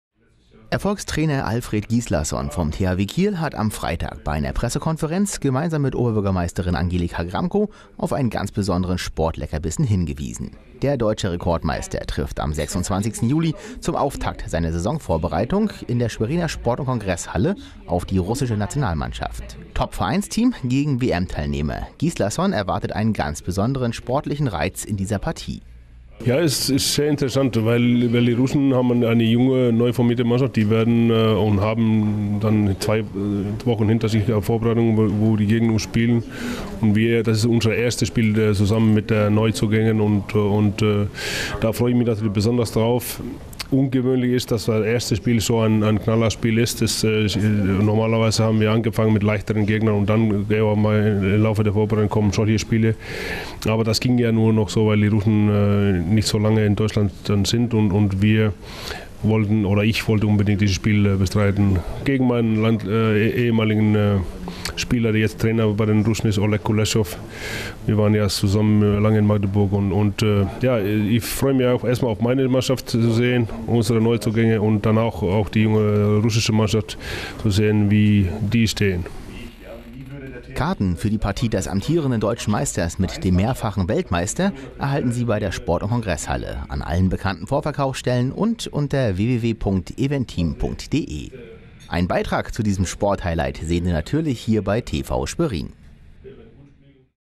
THW Kiel vs Russland in Schwerin
Alfred Gislason blickt mit Vorfreude auf Handball-Highlight